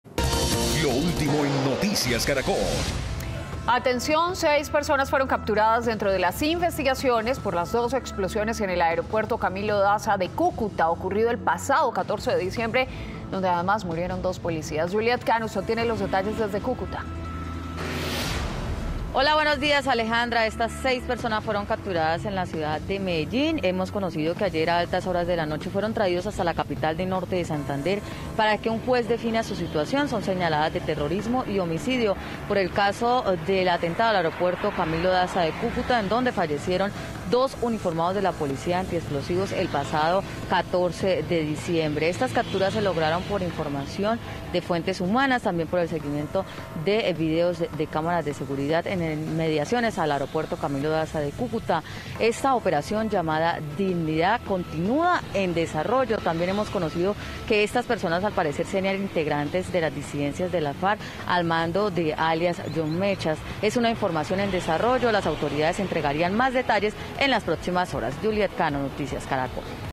Capturan a 6 presuntos responsables del atentado en aeropuerto de Cúcuta
Las autoridades lograron la captura de seis personas que habrían participado en el atentado con explosivos que se registró en el aeropuerto de Cúcuta y que dejó dos uniformados de la Policía muertos.

Las detenciones se llevaron a cabo en la ciudad de Medellín.

Los capturados fueron trasladados hasta la capital de Norte de Santander y serán presentados ante un juez.